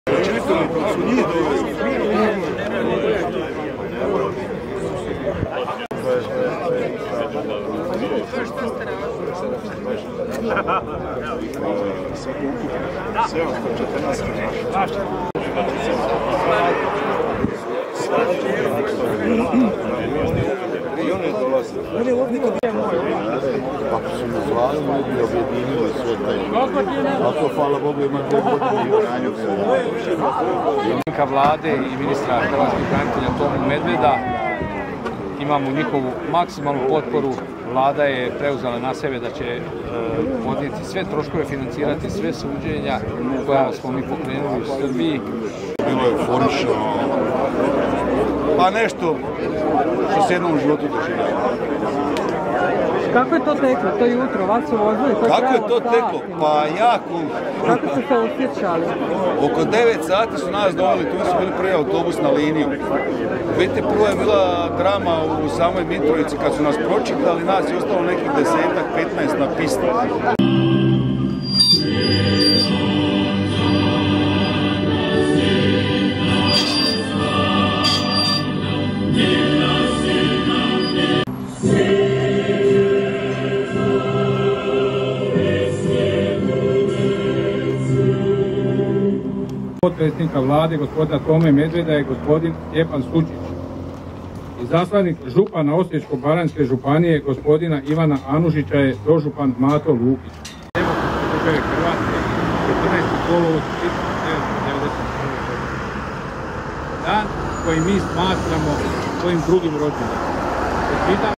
Dan sjećanja na zatočenike neprijateljskih logora u Domovinskom ratu u Nemetinu
Hrvatsko drustvo logorasa srpskih koncentracijskih logora obiljezilo je u Nemetinu Dan sjecanja na zatocenike neprijateljskih logora u Domovinskom ratu i razmjenu zatocenika logora.